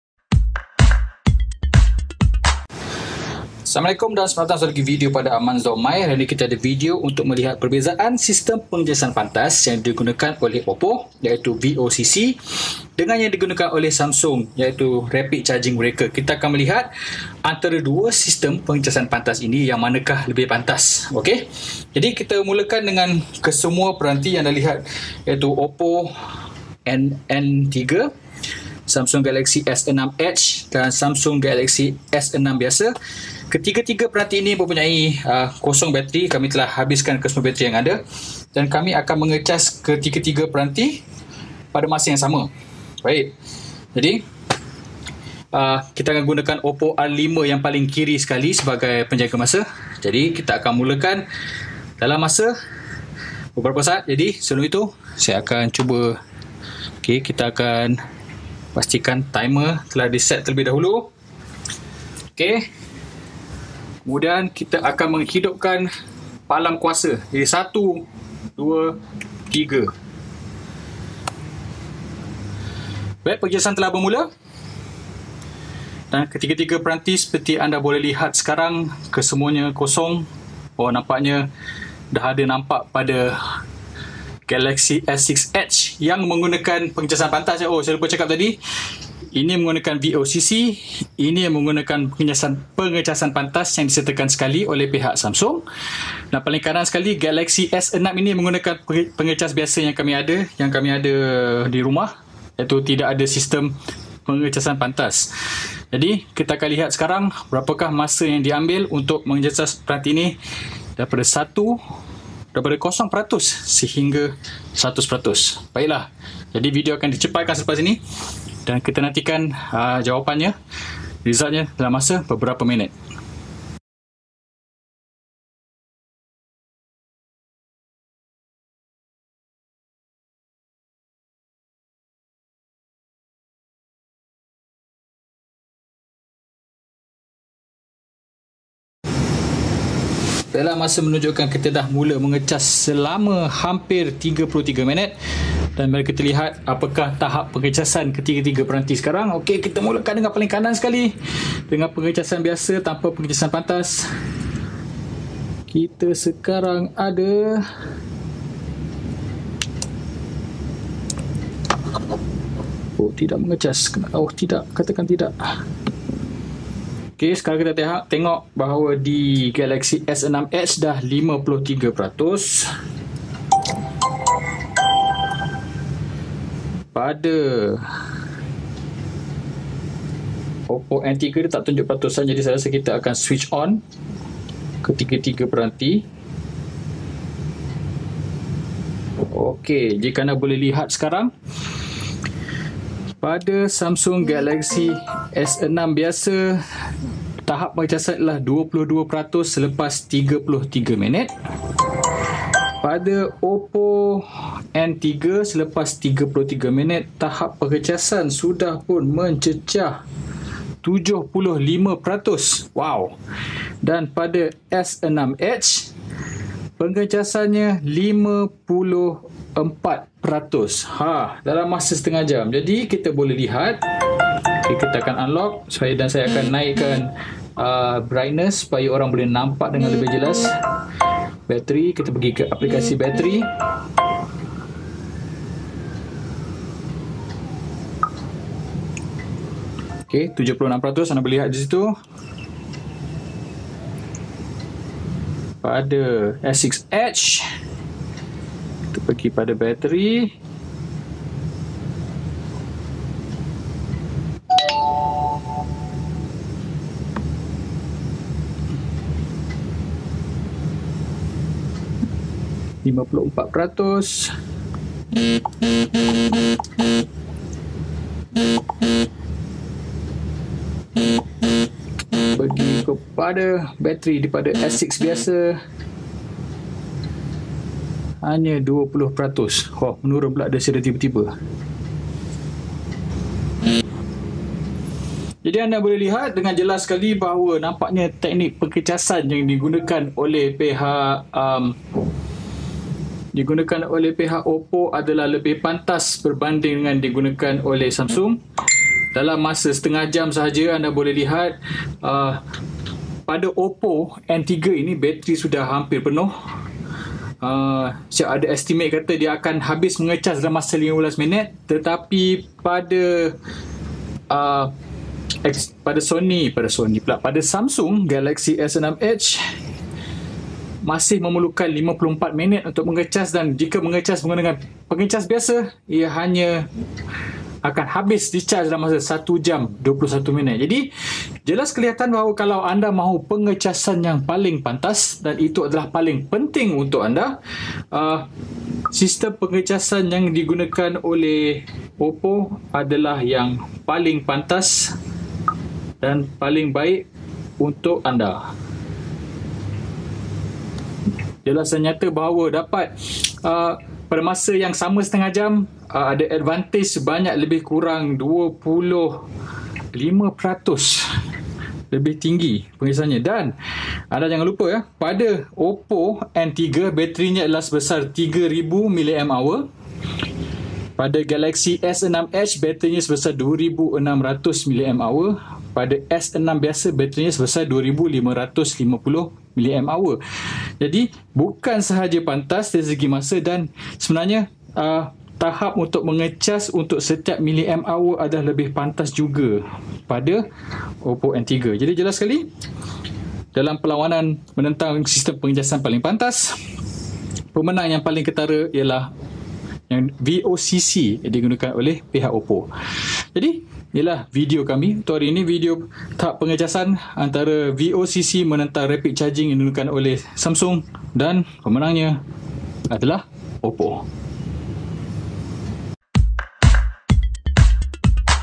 Perbezaan Sistem Pengecasan Pantas
Mari lihat perbezaan masa yang diambil untuk mengecas peranti menggunakan sistem VOOC Oppo dan Adaptive Rapid Charging  Samsung. Yang manakah boleh mengecas dengan lebih pantas?